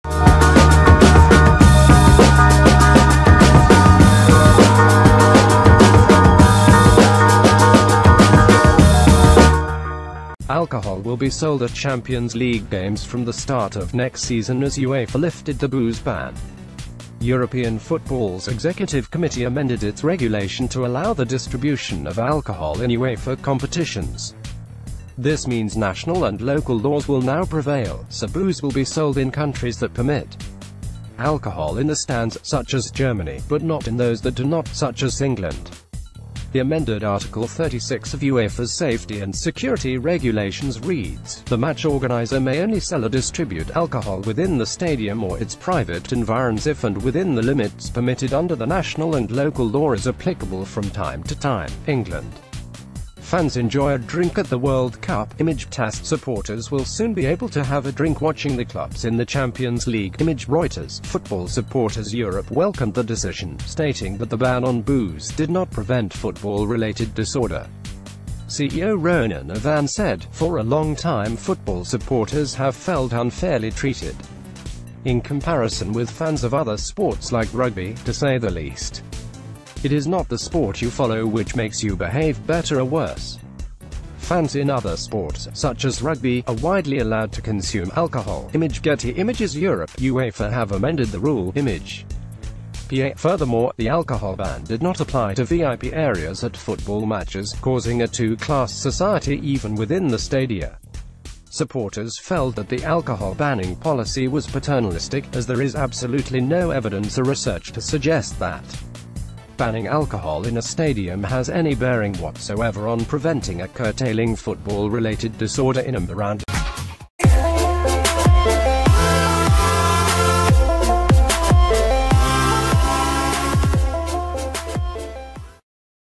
Alcohol to be sold at Champions League games as UEFA lift booze ban
► Alcohol will be sold at Champions League games from the start of next season as UEFA lifted their bo...